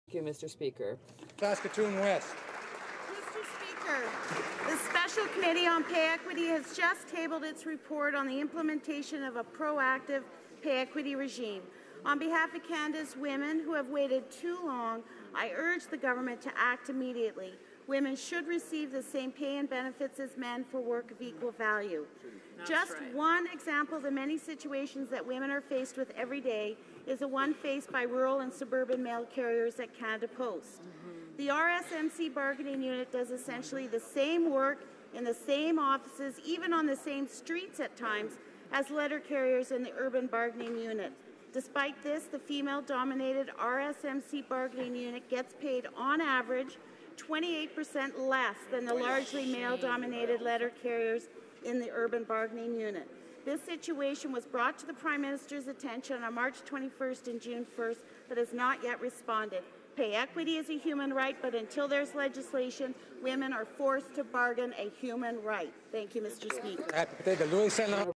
June 15 2016 - Pay Equity statement
Mr. Speaker, the Special Committee on Pay Equity has just tabled its report on the implementation of a proactive pay equity regime. On behalf of Canada's women, who have been waiting too long, I urge the government to act immediately. Women should receive the same pay and benefits as men for work of equal value.

Just one example of the many situations that women are faced with every day is the one faced by rural and suburban mail carriers at Canada Post. The RSMC bargaining unit does essentially the same work, in the same offices, even on the same streets at times, as the letter carriers in the urban bargaining unit.

Despite this, the female-dominated RSMC bargaining unit gets paid, on average, 28% less than the largely male-dominated letter carriers in the urban bargaining unit. This situation was brought to the Prime Minister's attention on March 21 and June 1, but he has not yet responded.

Pay equity is a human right, but until there is legislation, women are forced to bargain their human rights.